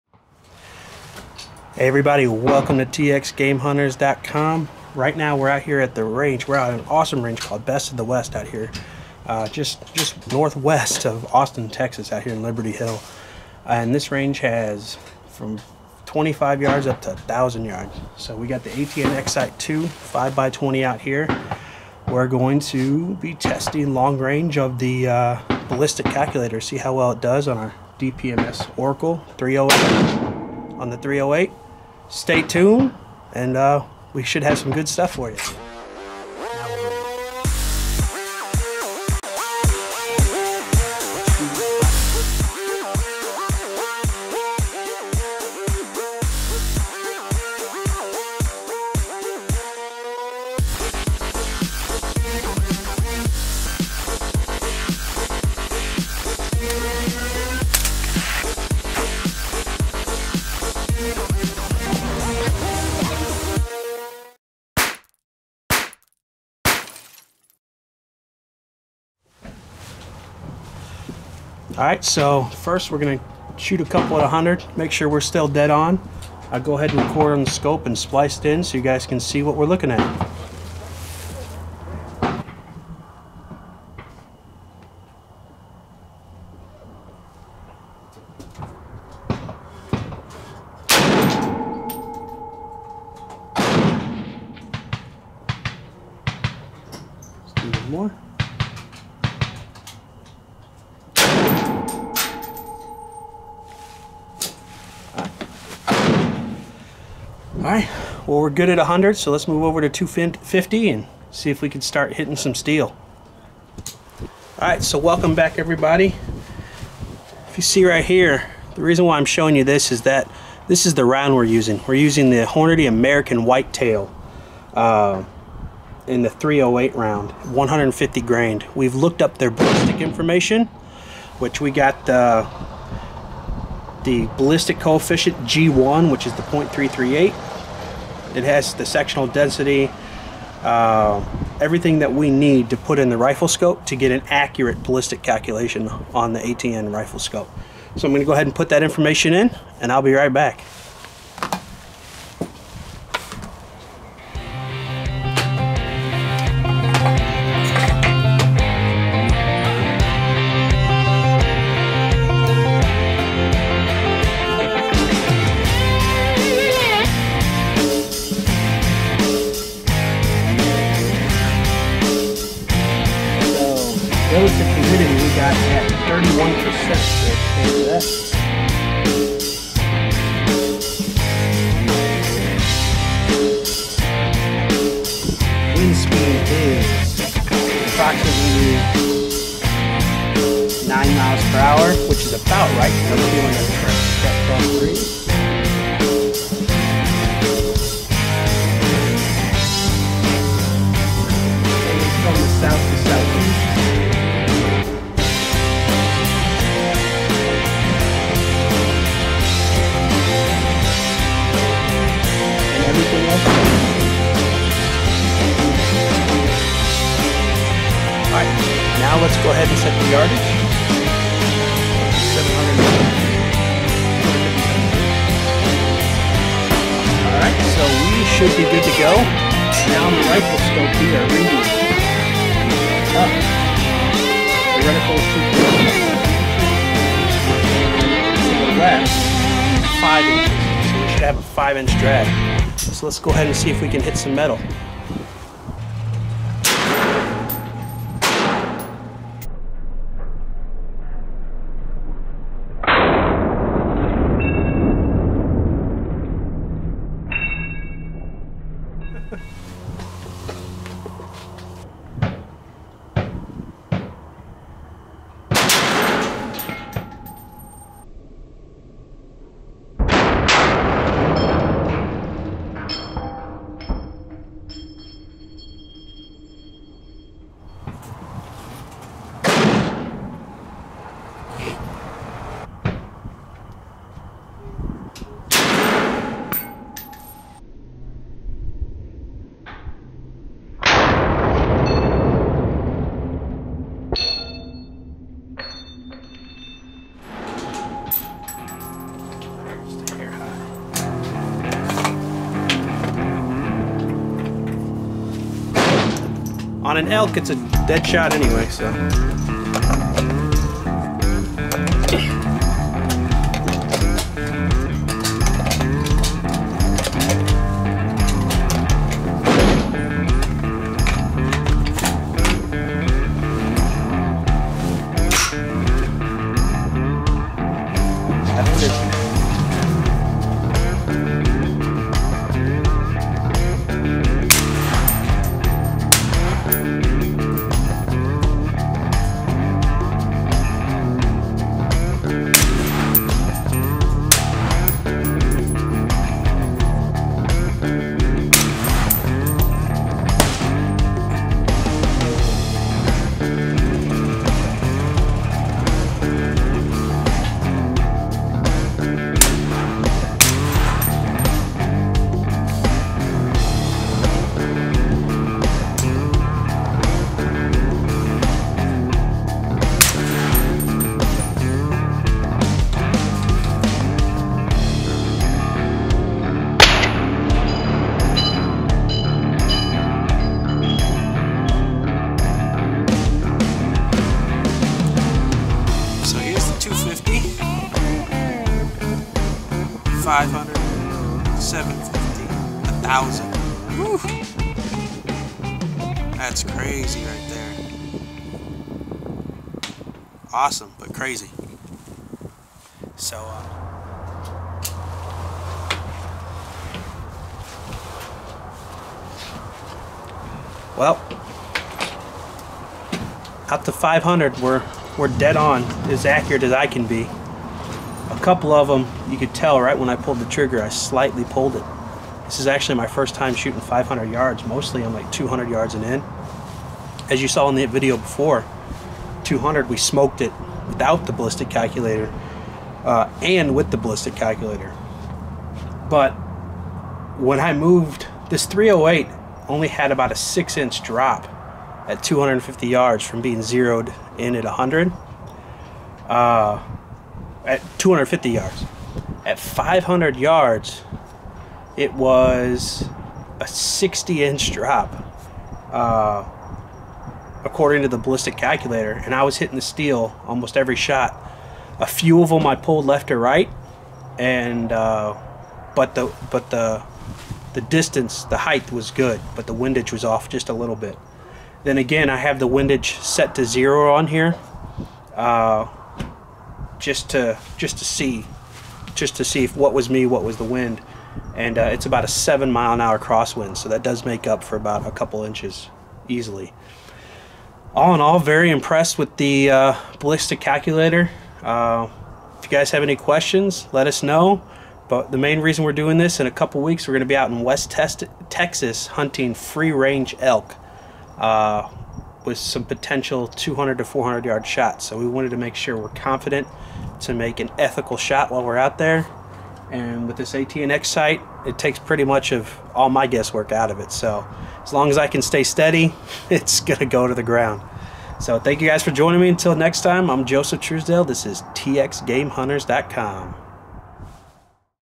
ATN X-Sight II 5x20 Ballistic Calculator 500 yards - WORKS GREAT!
Using the ATN X-Sight II 5x20 to test out the ballistic calculator up to 500 yards. Works great! DPMS Oracle .308 - Impressed!